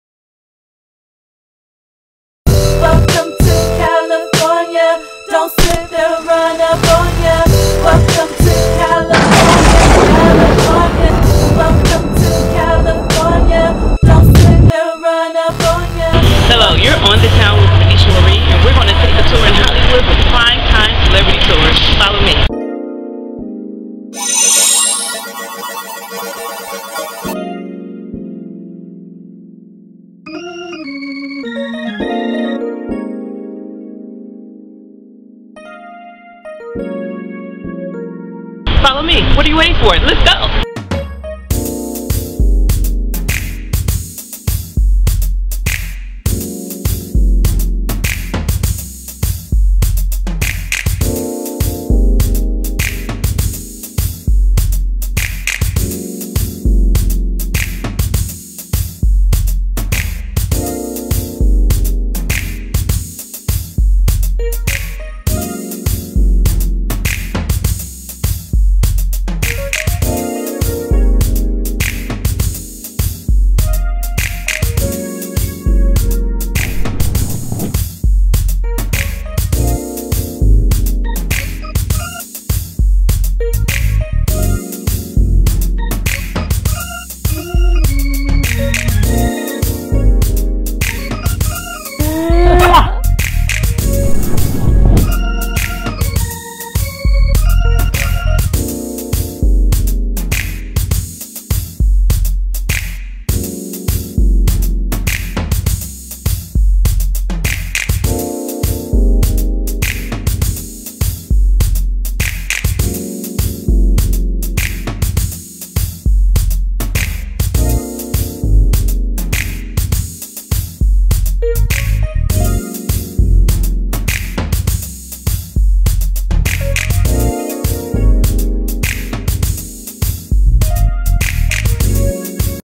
On the Town with Renisha Marie: Prime Time Hollywood Tours
Follow Renisha Marie on the town as she takes you on a fun and exciting journey throughout Los Angeles California.

Author of Limitless Inspirations

Renisha Marie Entertainment LLC.